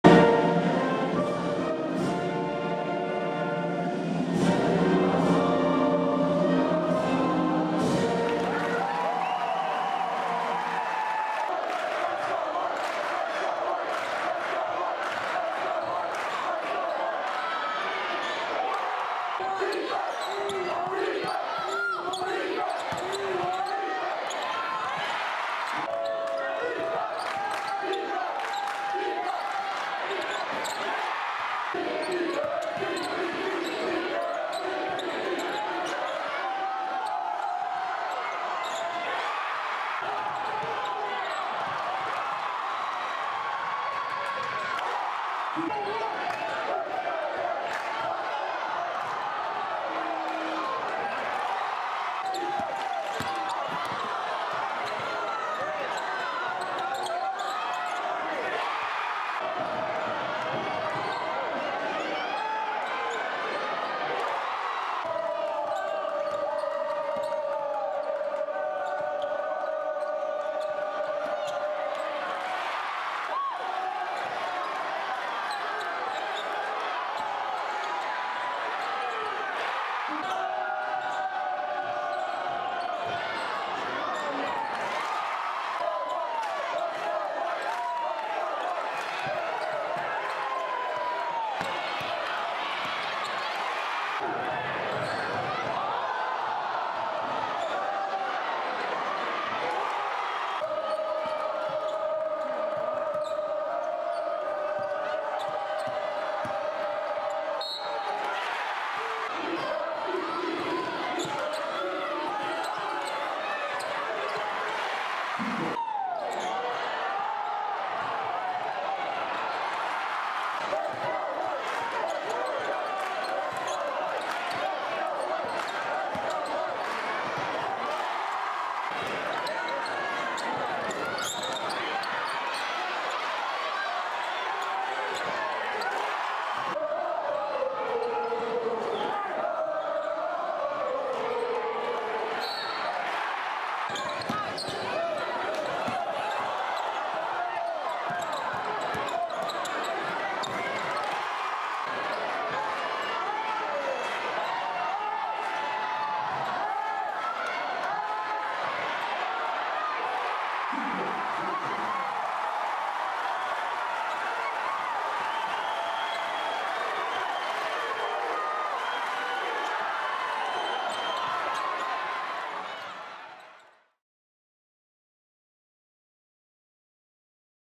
Arcadia vs. Ottoville Division IV Girl's Regional Finals
Arcaidia hangs in as long as they can againt Ottoville but ends the night with a loss, 52-37.